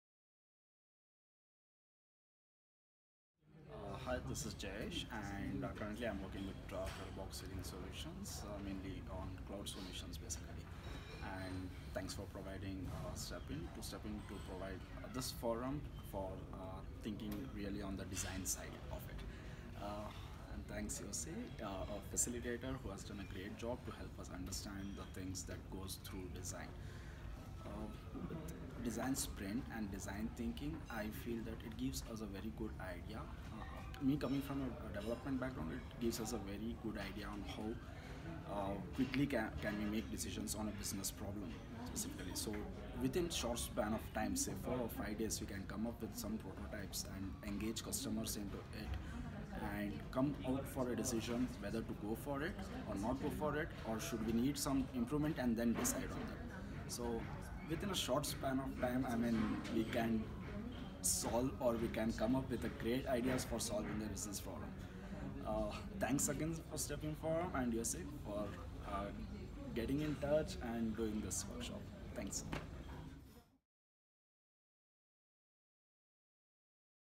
Design Sprint Master Training | Jayesh Gandhi
Good words by Jayesh Gandhi, Development Lead Cloud Solutions, Trelleborg who was one of our participants in the Design Sprint Workshop held on June 07 & 08, 2018 at Hyatt, Bangalore